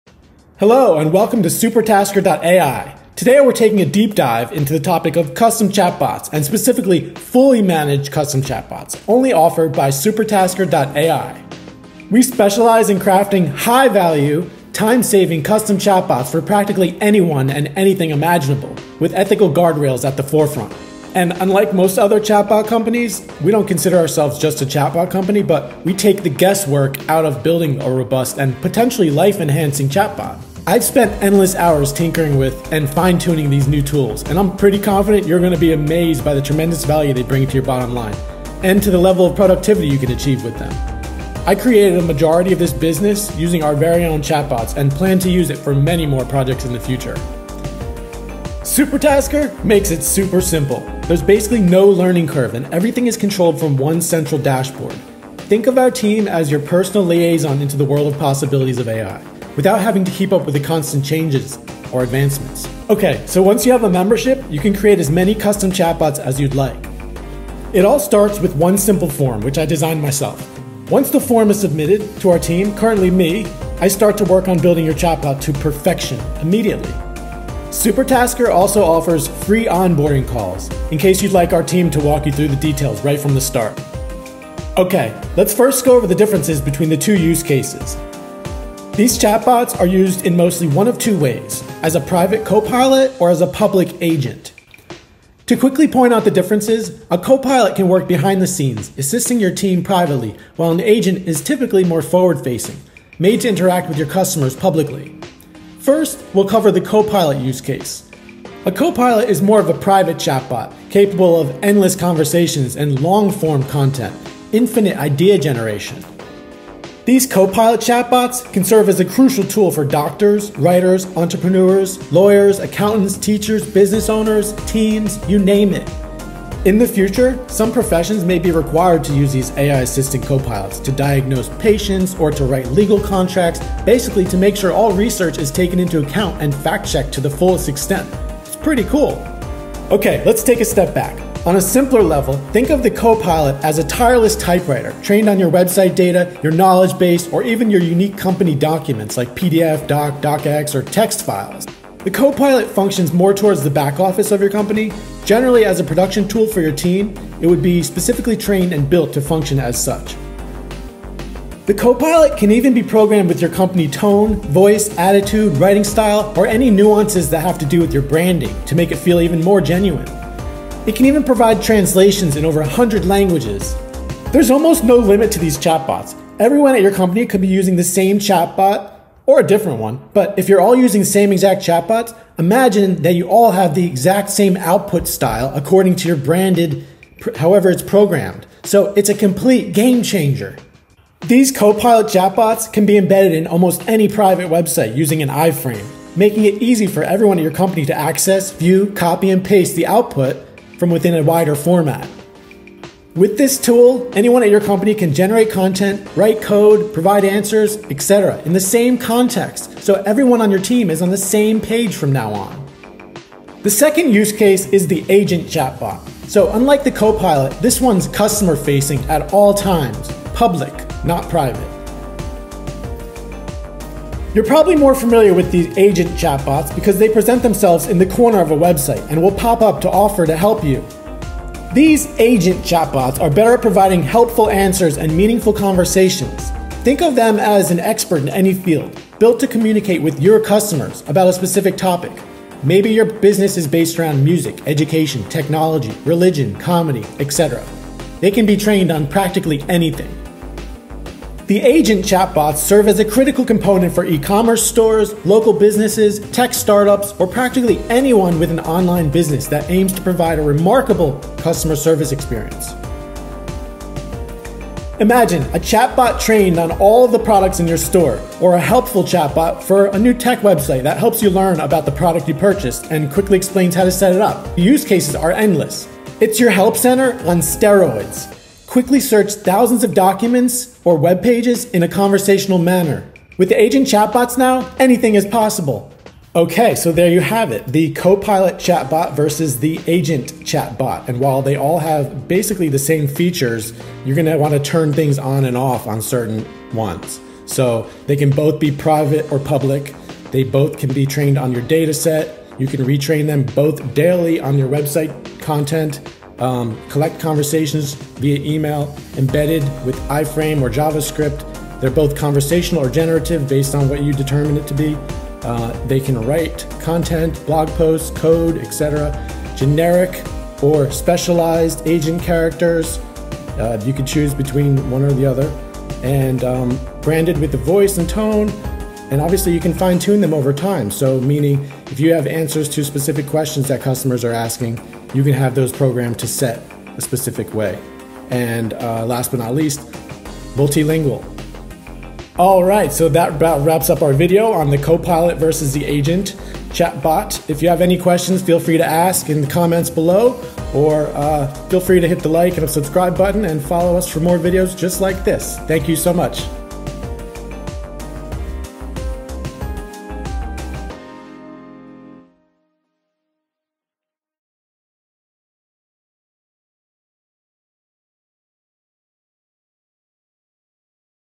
🔥 Chatbots Explained! Part 1 • Copilot vs. Agent Custom Chatbot Features
Dive into Supertasker.ai's detailed breakdown of chatbot technology! Discover the nuances between Copilot and Agent custom chatbots, learn their unique features, and find out how they can revolutionize communication for businesses and individuals alike. A must-watch for anyone eager to harness the power of advanced AI chatbots!